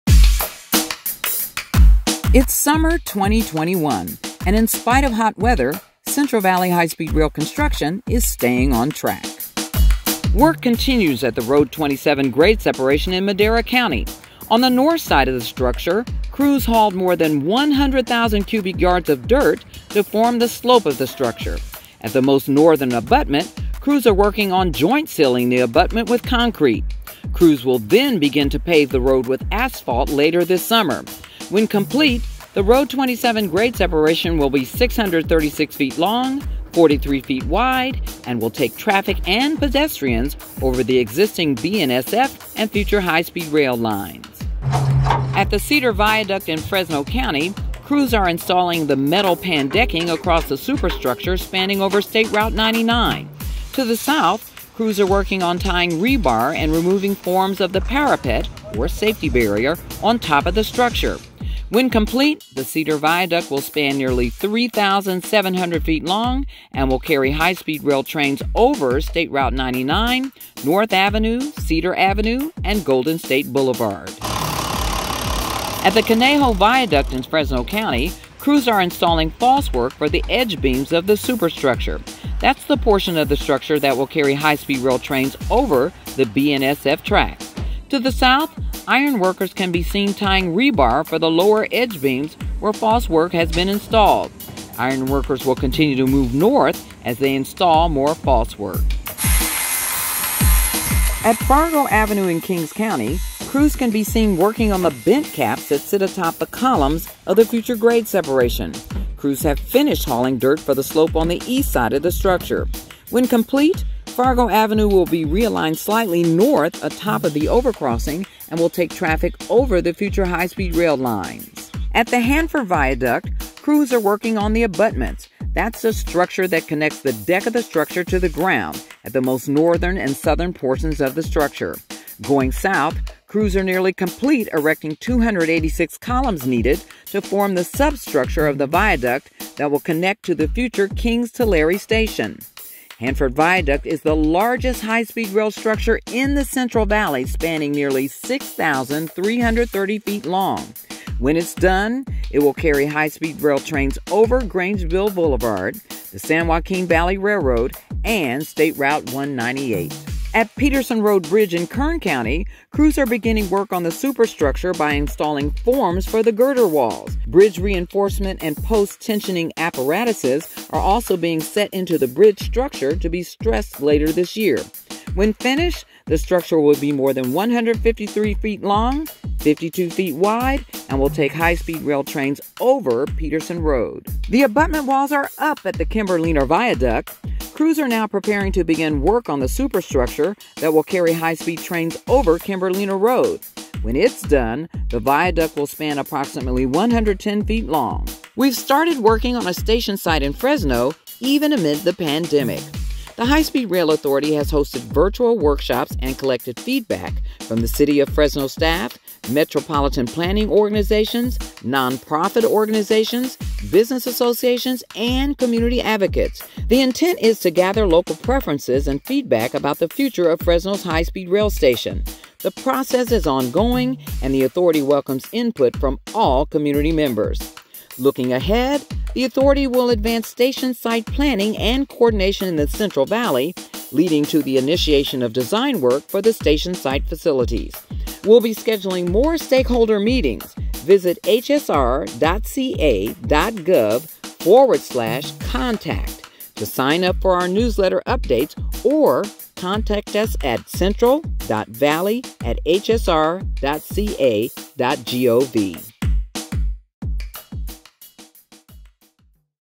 California High-Speed Rail Construction Update - Summer 2021
The California High-Speed Rail Authority’s August 2021 Construction Update highlights the progress being made on the nation’s first high-speed rail project. Highlights include summer progress across all high-speed rail construction packages and engagement with Fresno stakeholders as we plan for the future Fresno high-speed rail station. The Authority currently has 119 miles under construction in the Central Valley within three construction packages. Design-builder contractors Tutor-Perini/Zachry/Parsons, Dragados-Flatiron/Joint Venture, and California Rail Builders together have 35 active construction sites with an average of 1,100 workers dispatched each day.